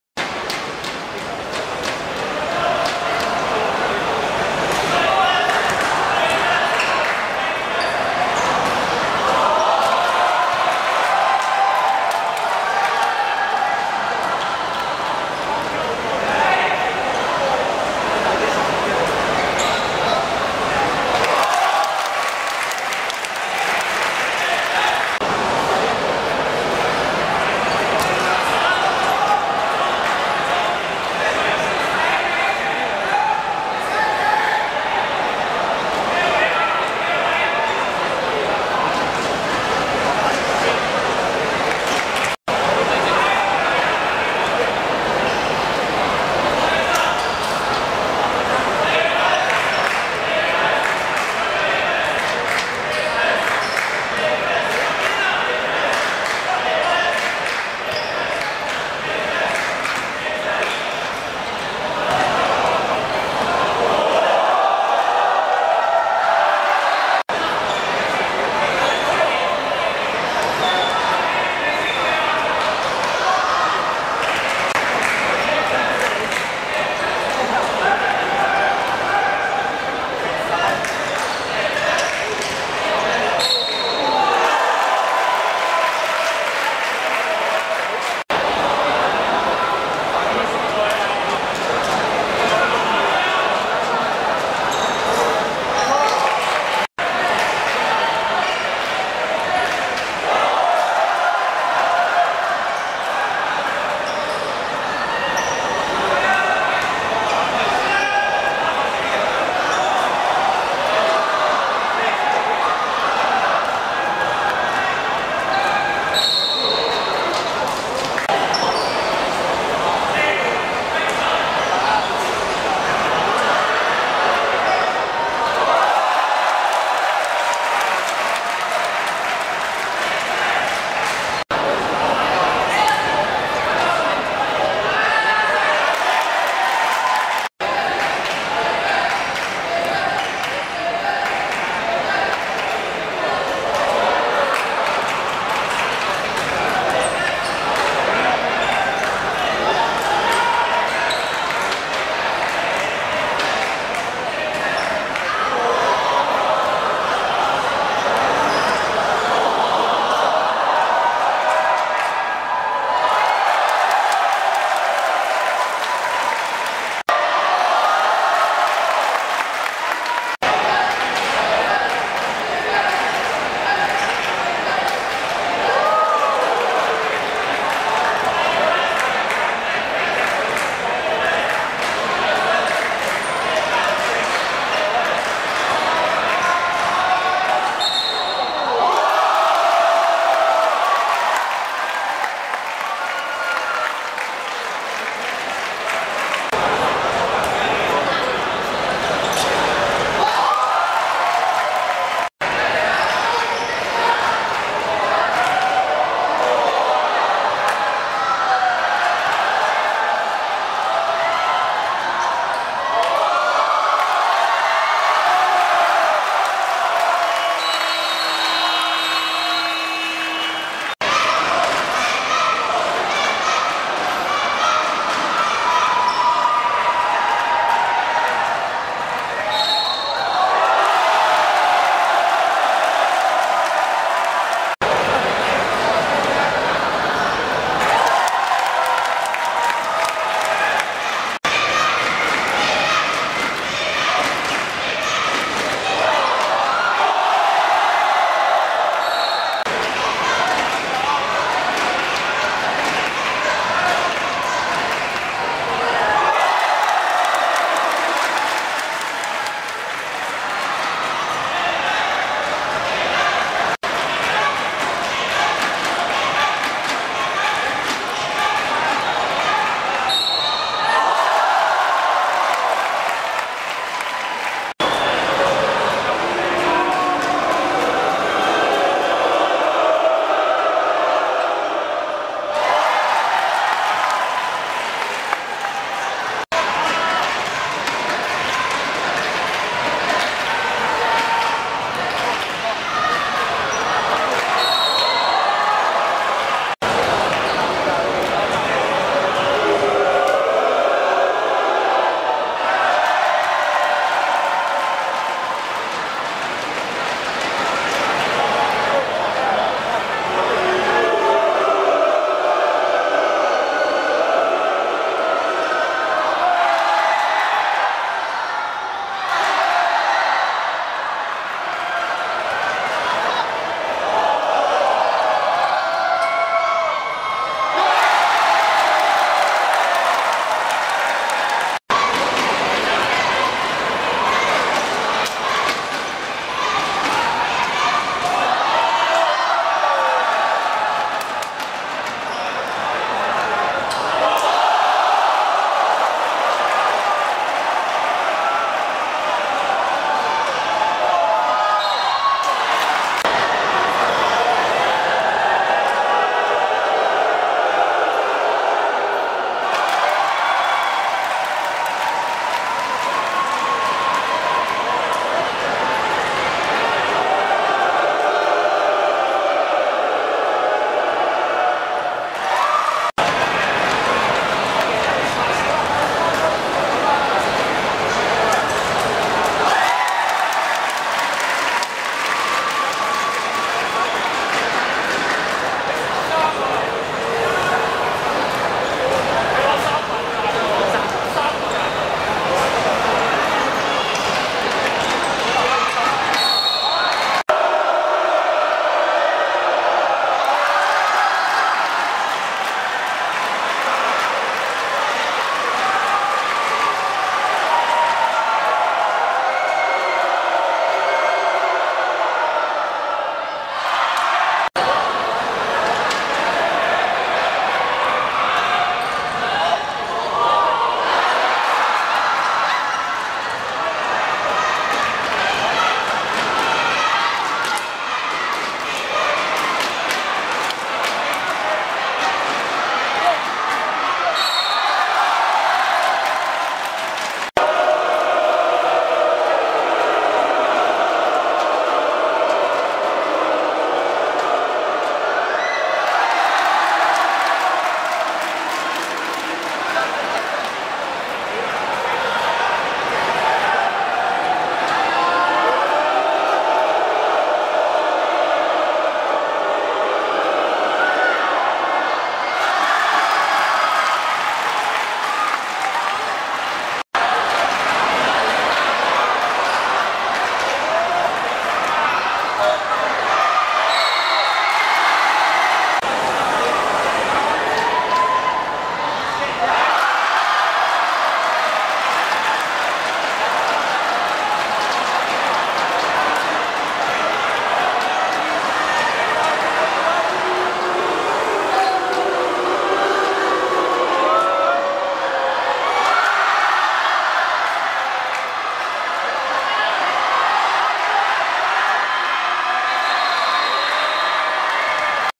天蝠vs翼龍(Nike League 2012~u16男子決賽)第四節精華
2012.8.4.灣仔修頓室內塲 15:30 法定時間(66:66)7度平手 最高分數拉開14分 進入加時 天蝠領先 但為翼龍追過 最後反擊犯下基本錯誤 錯失扳平機會 翼龍以76:74奪得2012 NIKE LEAGUE U16組別冠軍